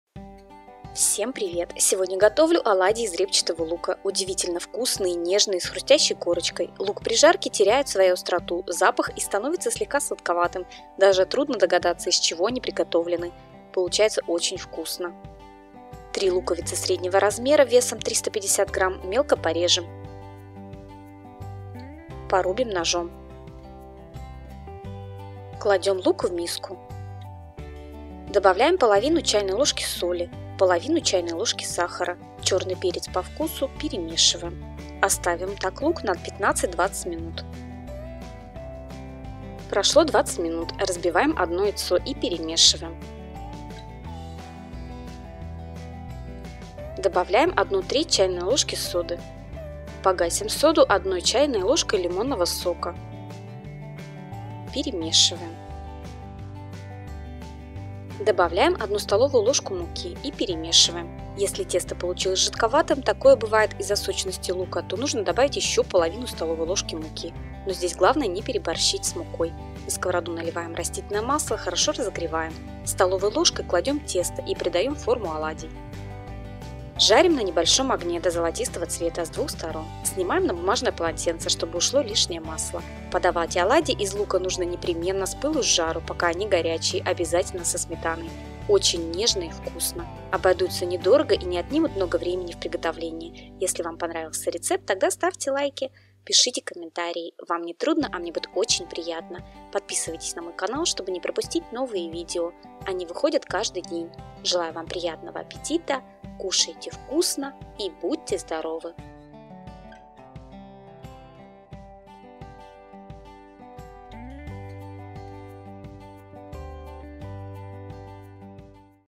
Приготовила на завтрак необычные оладьи, даже не думала, что мой ребенок добавки попросит!
Оладьи из репчатого лука. Удивительно вкусные, нежные с хрустящей корочкой. Лук при жарке теряет свою остроту,  запах и становится слегка сладковатым, даже трудно догадаться, из чего они приготовлены . Получается очень вкусно. Обойдутся не дорого и не отнимут много времени в приготовлении

Рецепт:
Лук – 3шт( 350гр.)
Соль- 1/2ч.л.
Сахар -1/2ч.л.
Черный перец -  по вкусу
Яйцо – 1шт.
Мука – 1-1,5ст.л.
Сода – 1/3ч.л.
Сок лимона – 1ч.л.